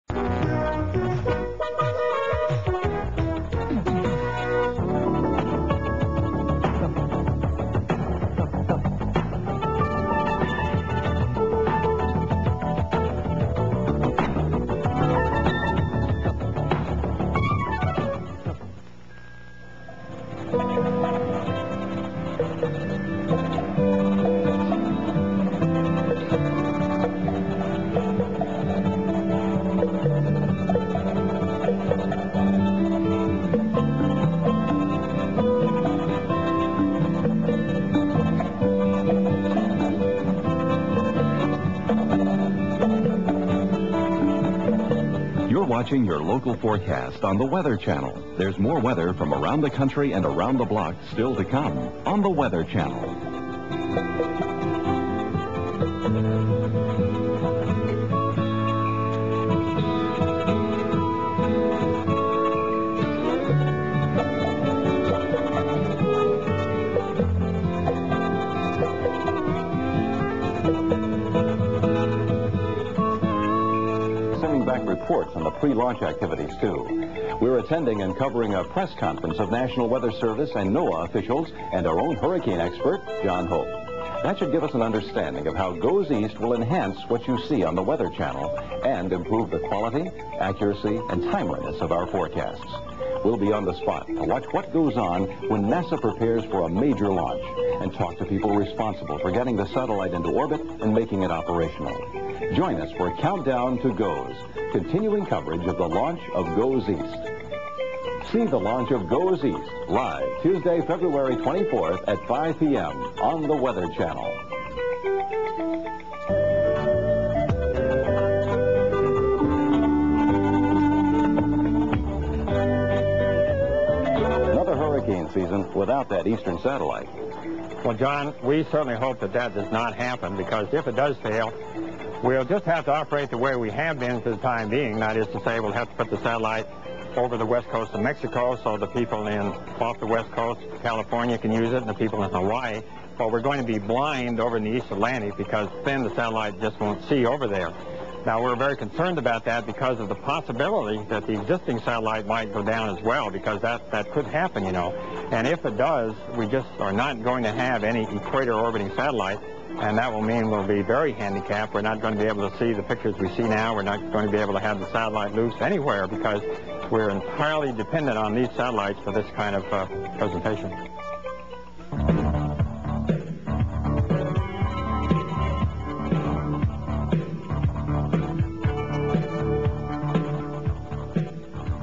Countdown to GOES East, February 1987
The Weather Channel prepares for the launch of GOES East.  John Hope explains.  Video for TWC Classics, Courtesy Craig Lehan.